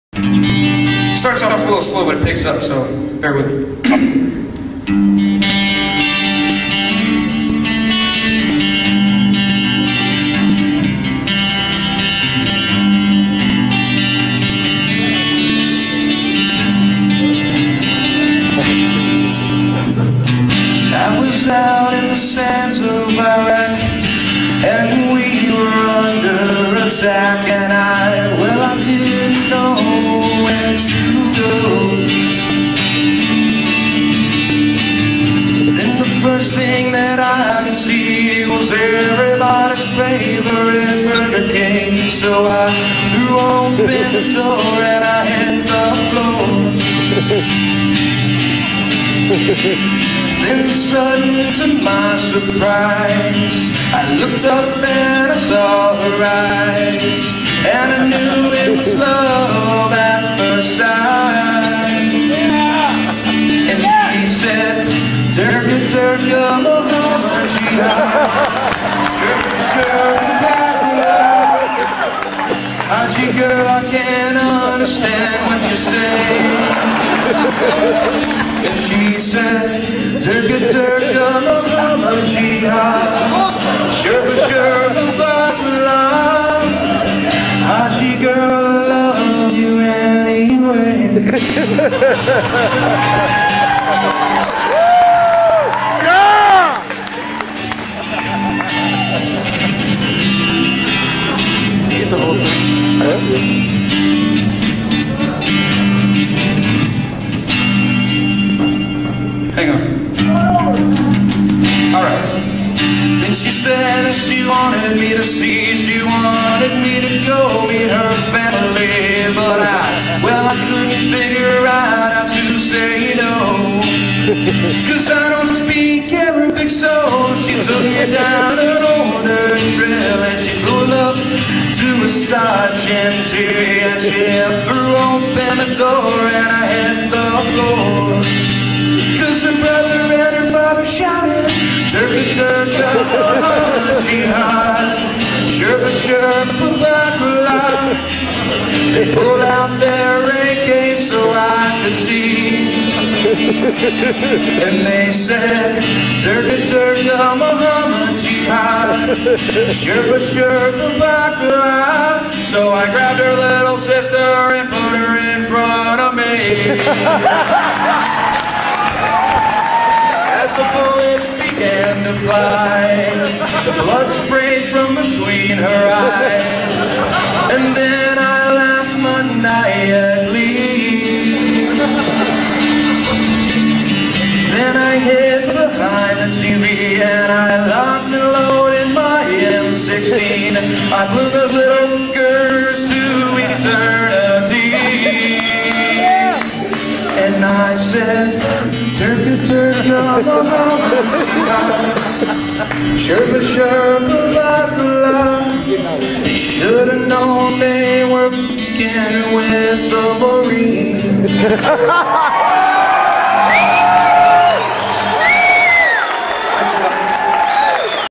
marine song
a US MArine sings a pretty song.